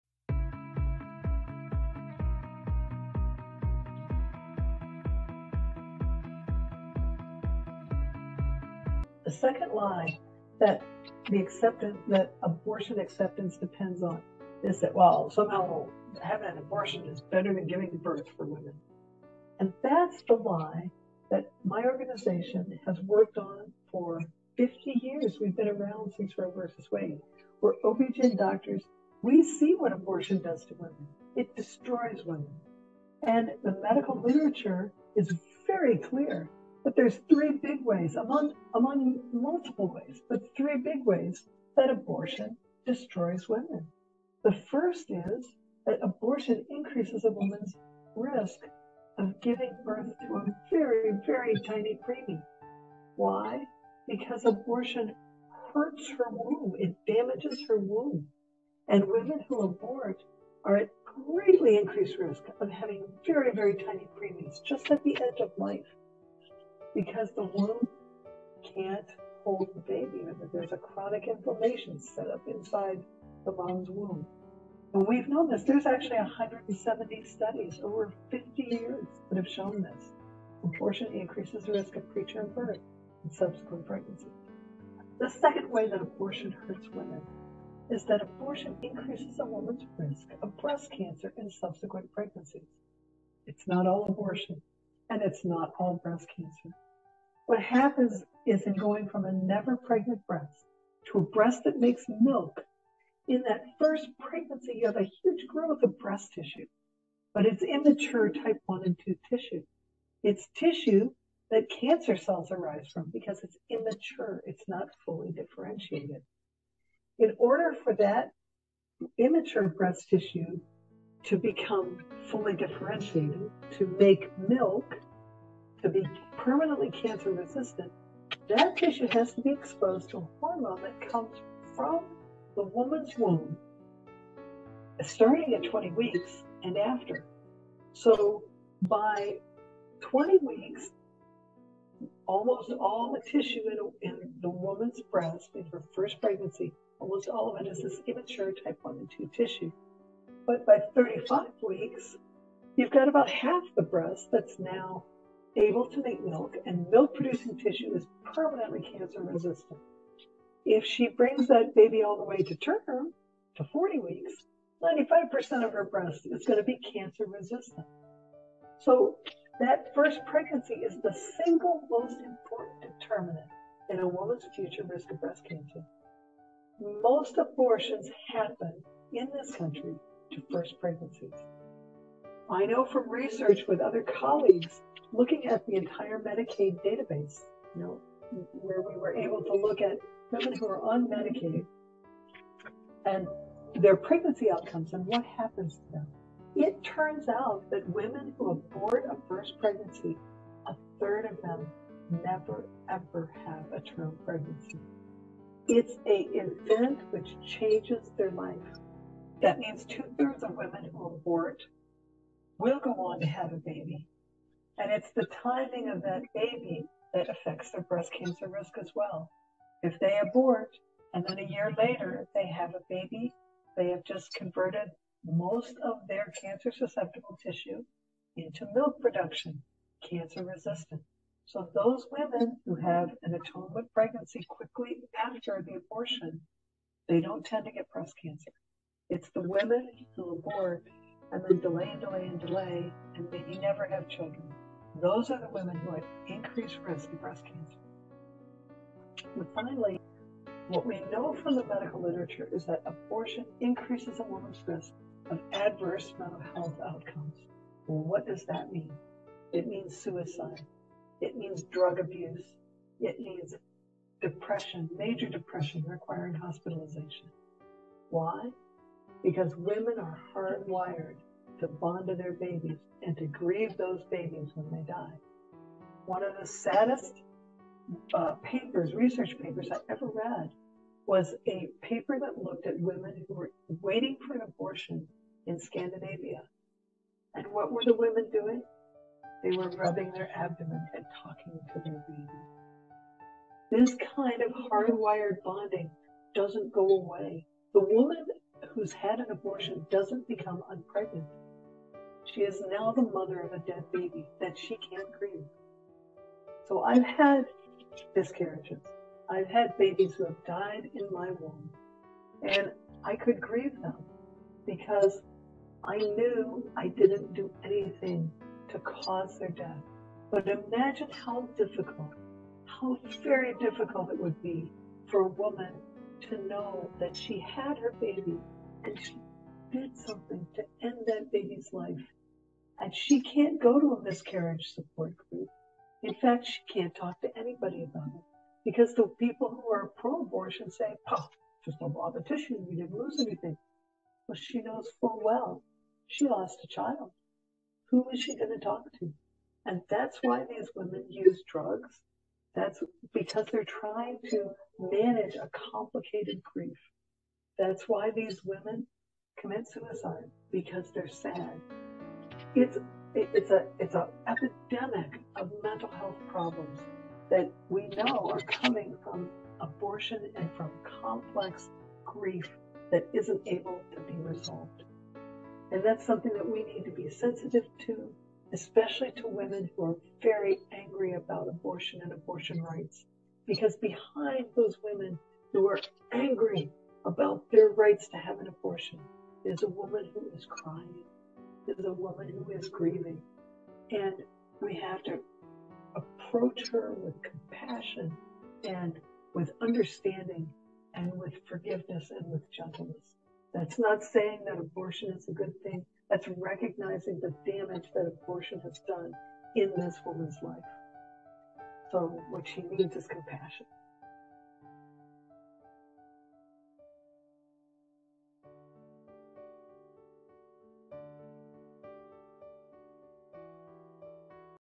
Sanctity of Human Life Moment: Responding to the argument that abortion is better than giving birth
In this video, Donna Harrison, MD., discusses the myth that abortion is better for a woman than giving birth.